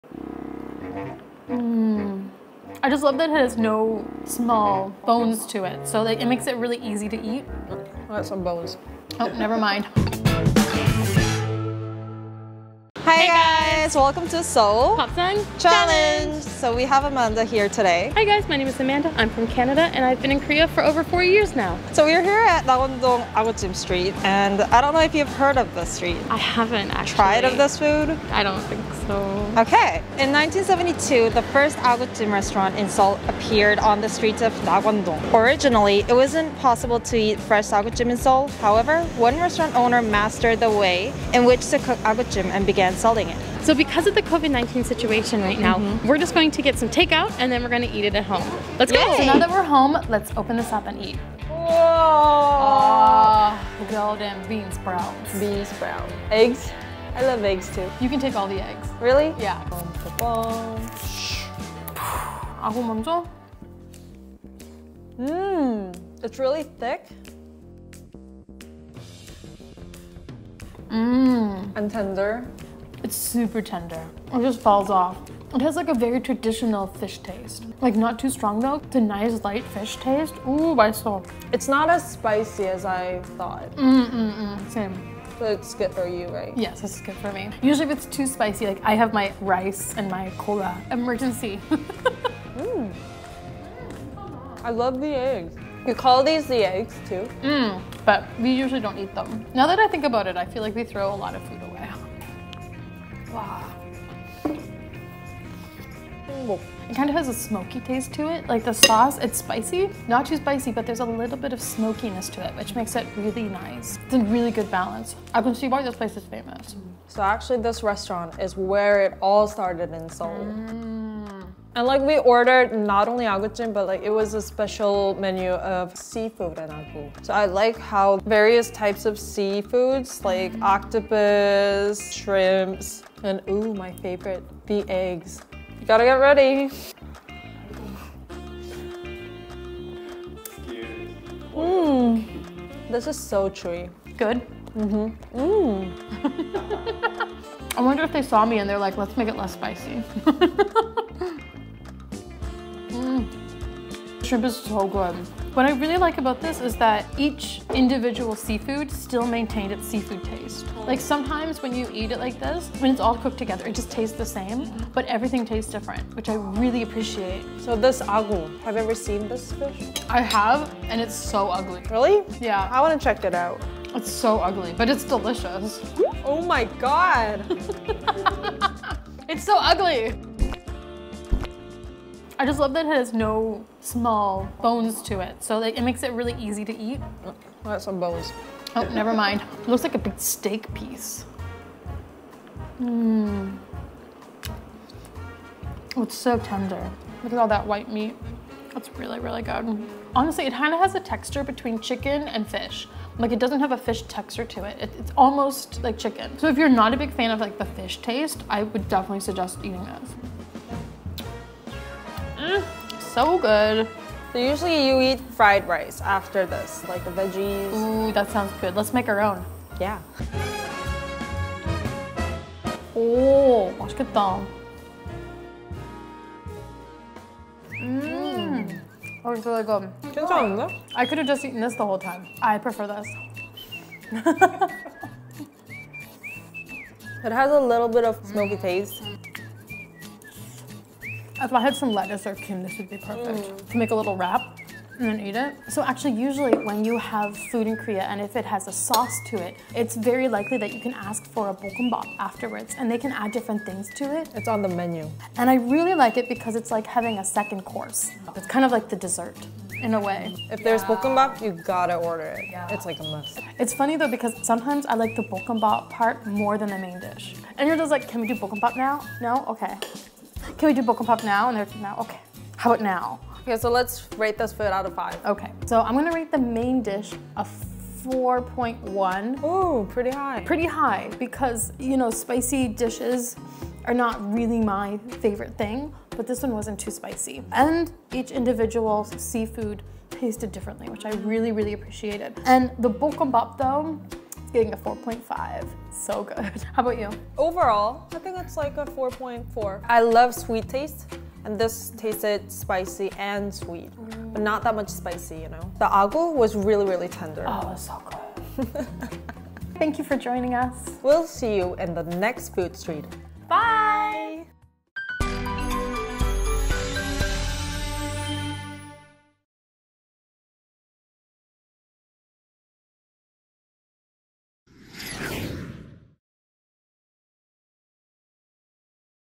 캐나다에서 온 아만다와 낙원동 아귀찜 먹방
이번 편에서는 낙원동 아귀찜을 포장해와서 먹방을 진행했습니다.
외국인과 함께 맛보는 아귀찜의 맛! 외국인 친구의 반응은 어떨까? 맛있을까?
영상을 통해 확인하시죠!

외국인의 입장에서 서울에서 먹어 볼 수 있는 다양한 음식을 직접 체험하여 리뷰하는 로드 버라이어티!
친숙한 음식부터 한국인에게도 생소한 음식까지 지금 가장 핫한 서울 대표 음식 테마거리를 찾아다니며 먹거리를 리뷰하고 먹방을 진행합니다.

자막 제공 : 영어, 일본어, 중국어